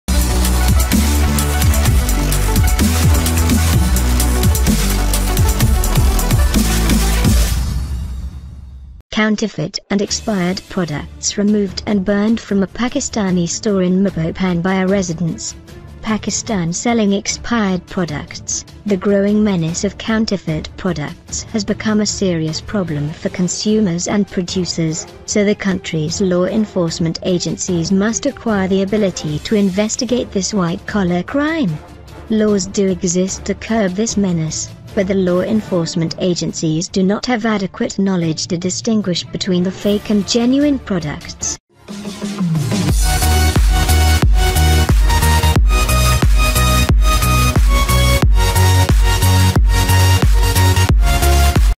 Counterfeit and expired products removed and burned from a Pakistani store in Mabopane
Counterfeit and expired products removed and burned from a Pakistani store in Mabopane by a residents. Pakistan selling expired products - The growing menace of counterfeit products has become a serious problem for consumers and producers, so the country’s law enforcement agencies must acquire the ability to investigate this white-collar crime. Laws do exist to curb this menace, but the law enforcement agencies do not have adequate knowledge to distinguish between the fake and genuine products.
📽 Robert Mugabe at the un 2018